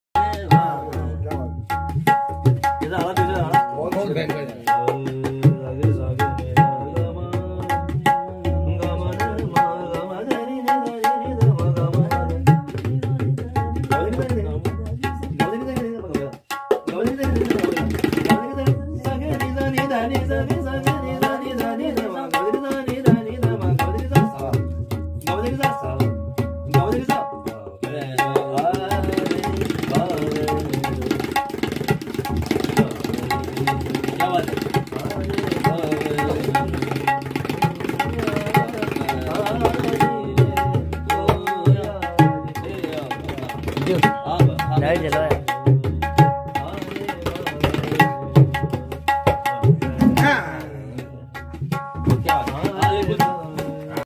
Toshi Sabri Shaarib Sabri Bollywood singer and music director tabla accompany mehraj Hussain
Chhota khyal teen Taal bandish